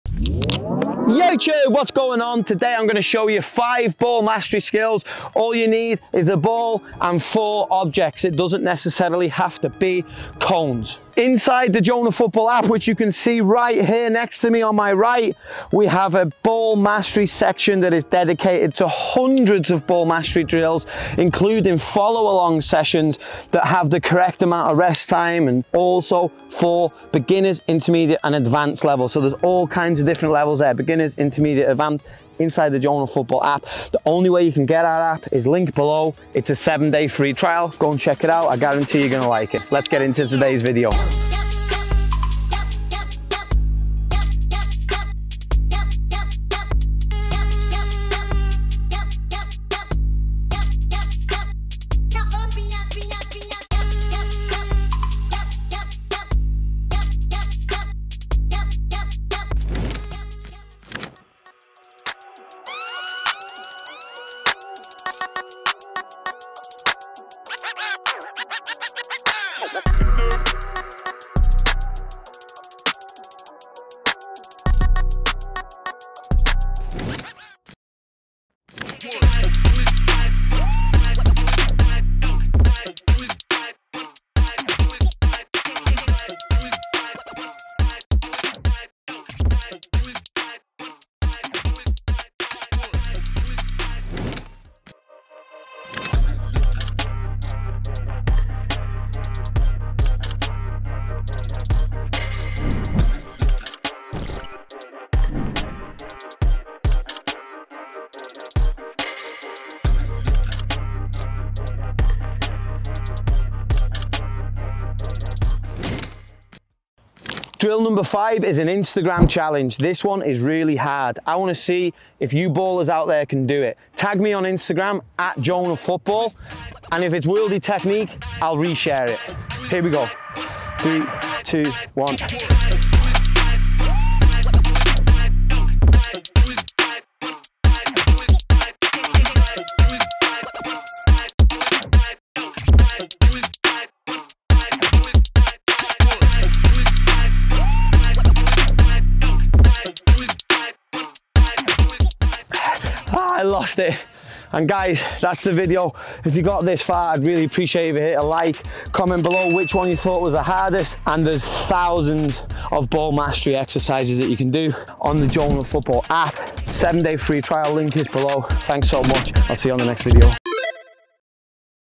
5 HARD BALL MASTERY DRILLS ⚽️ | JONER FOOTBALL
Today you will learn how to do 5 advanced 4 cone ball mastery drills! Can you do them all?

Timecodes:
00:41 - Drill One
01:00 - Drill Two
01:17 - Drill Three
01:34 - Drill Four
01:56-  Drill Five (Insta Challenge)